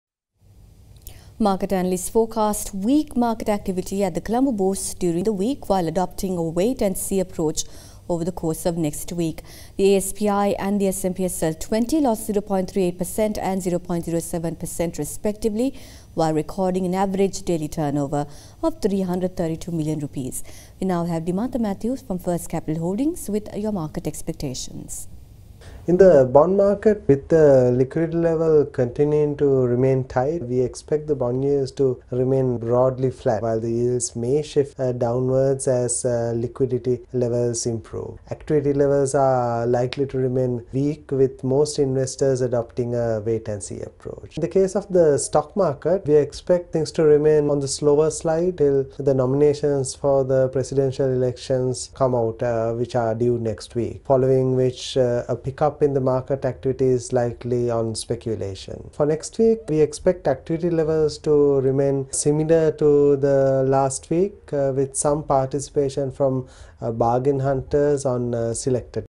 Dimantha Mathew at First Capital commenting on the bond and share market forecast 29.09.2019
Dimantha Mathew, Head of Research at First Capital commenting on the bond and share market forecast CSE CBSL lka.